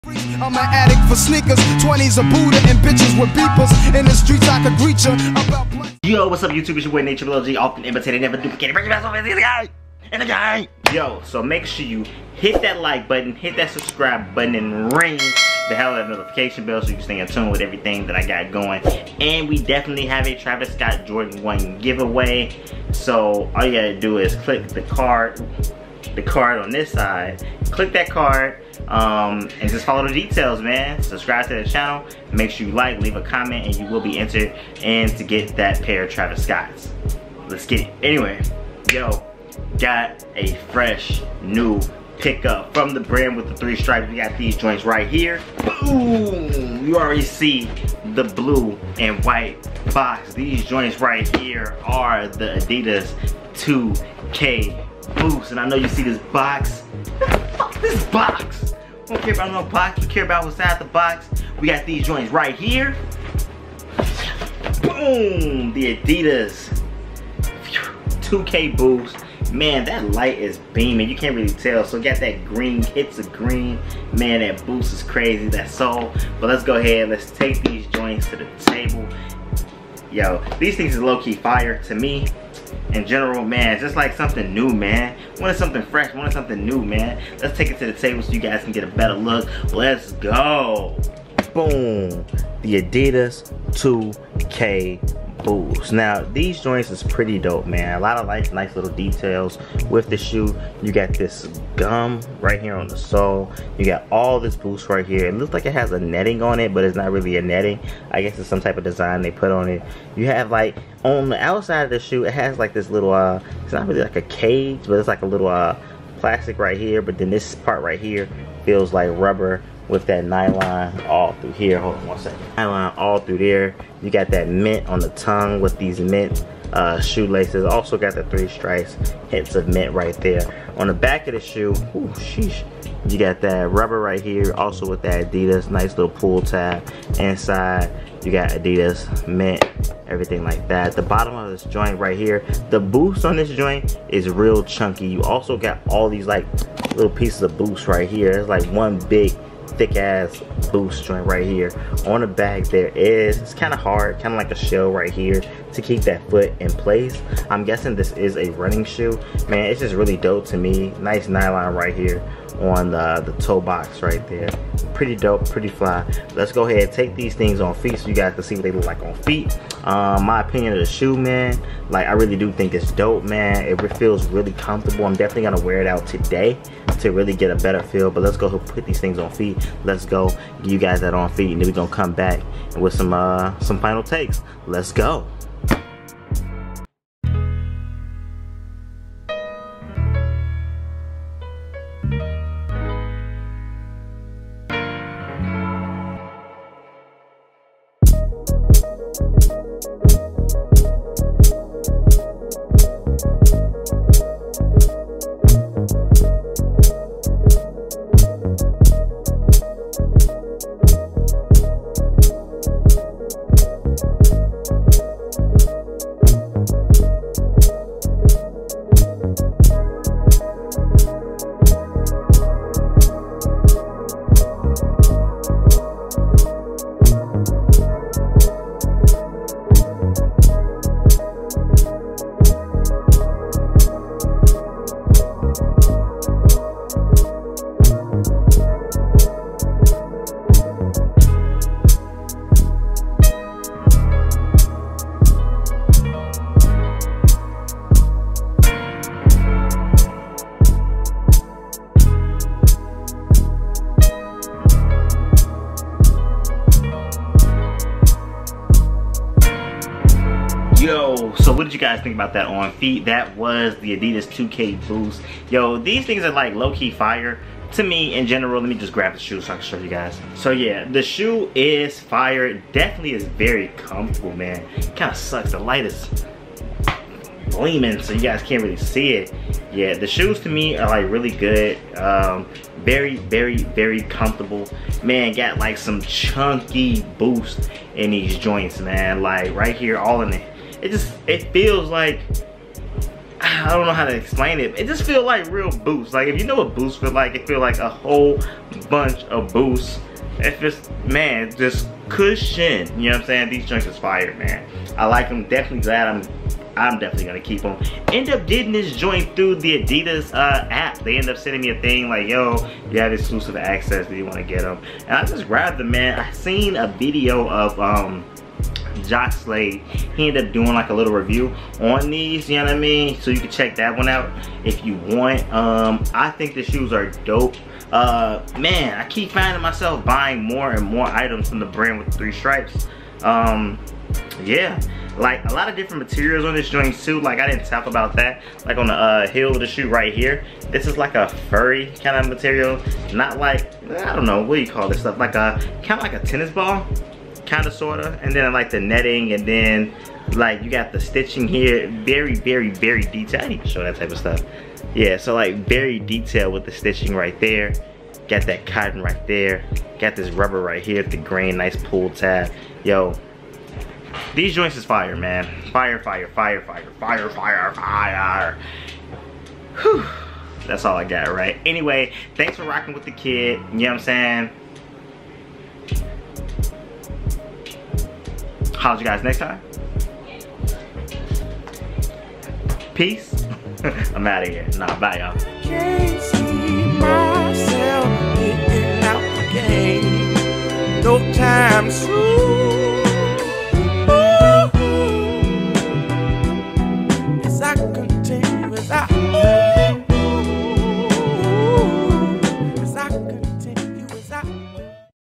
Adidas ZX 2K BOOST Review
Yo Whats good fam wanted to give you guys a fire review from the brand with the 3 stripes The ZX 2K Boost is the most comfortable boost sneaker ive ever put on! these joints are fire tell me your thoughts!!! do you wear everything? or only 1 brand?